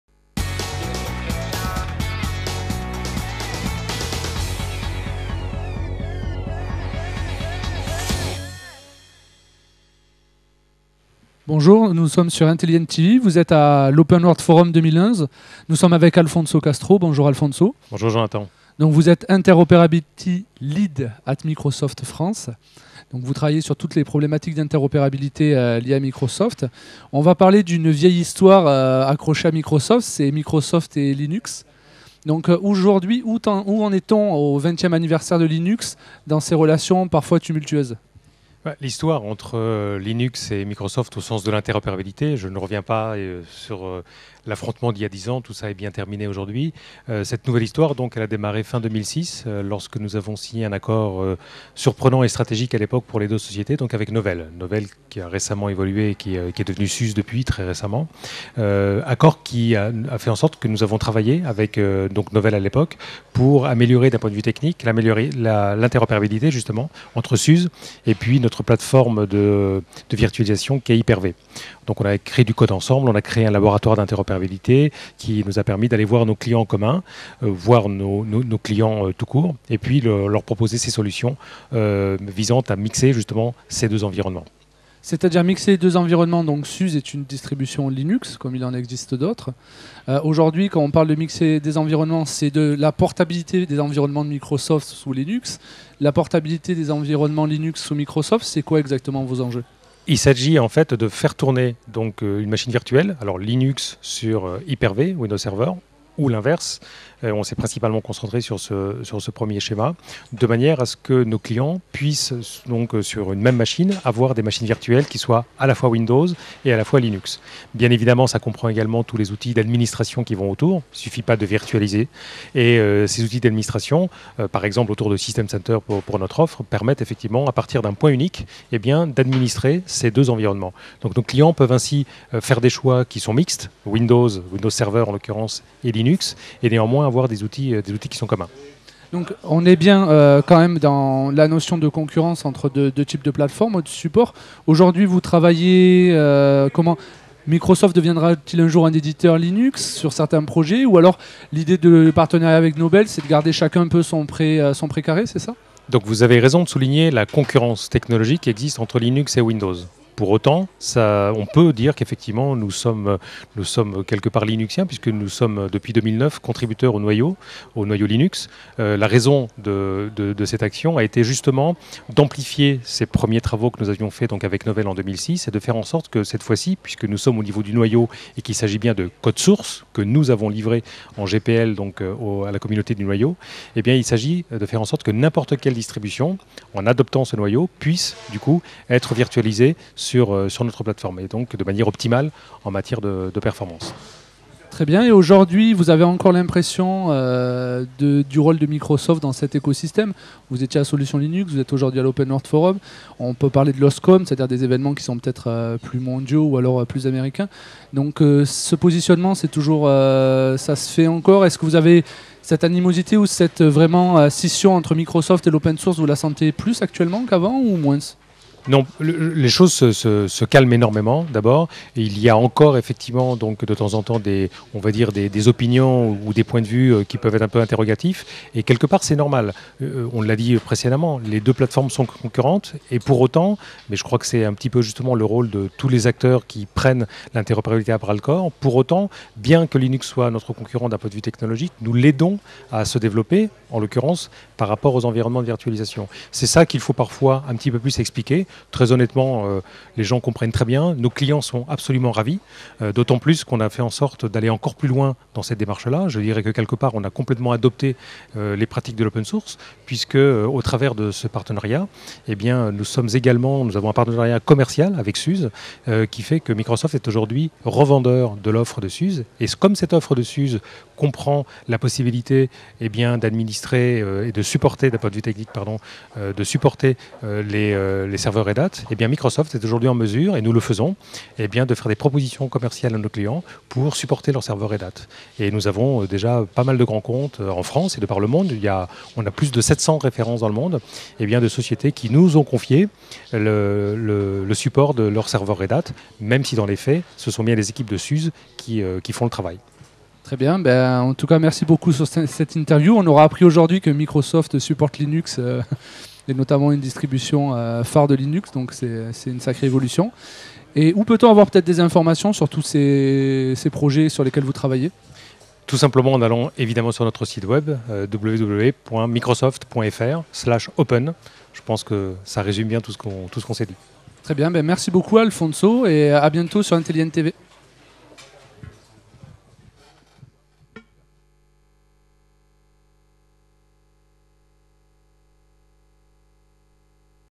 Azure et l'open source par Alfonso Castro, Interoperability lead at Microsoft France
Azure et l'open source par Alfonso Castro, Interoperability lead at Microsoft France.  Il est interviewé par Jonathan Le Lous - Open World Forum 2011 - Intelli'N TV